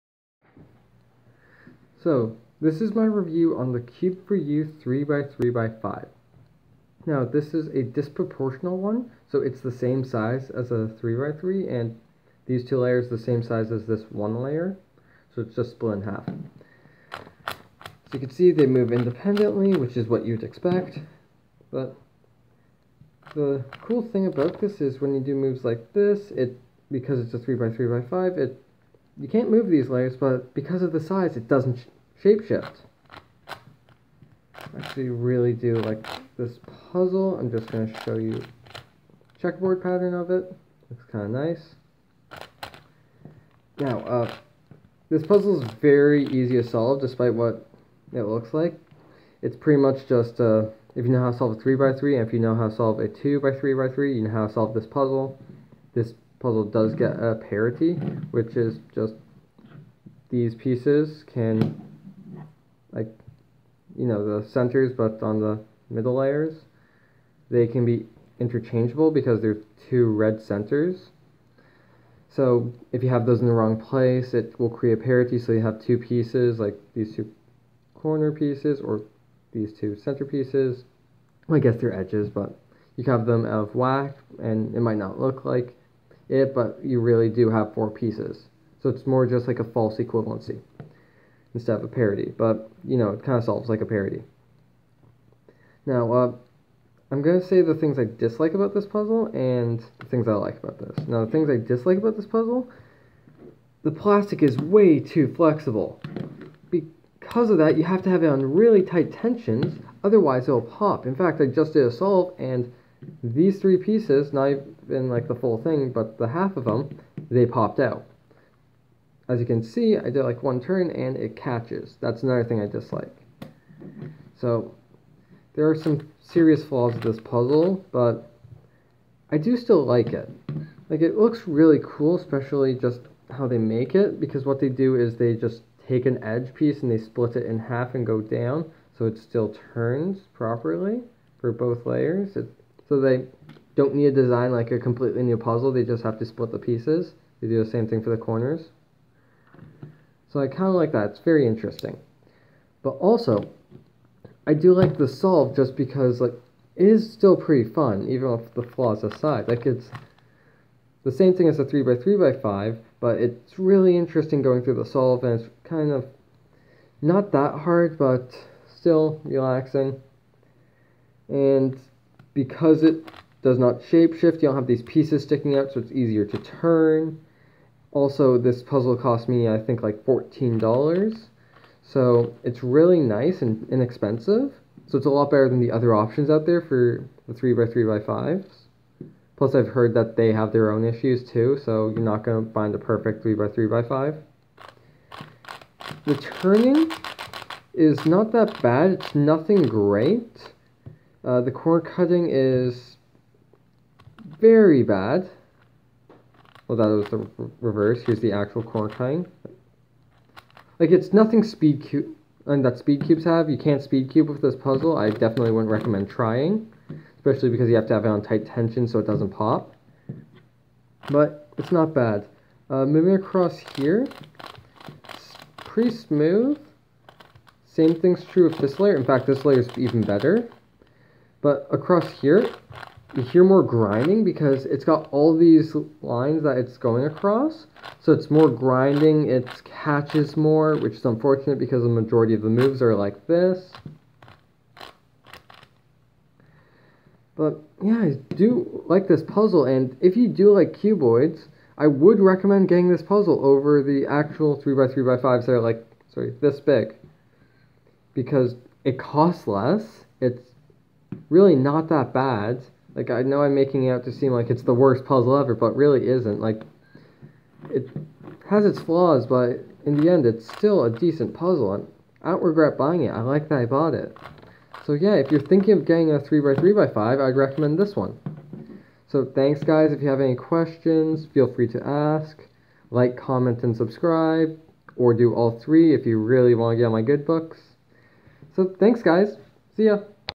C4U 3x3x5 Review
Disproportional 3x3x5. Because of this it is cubic.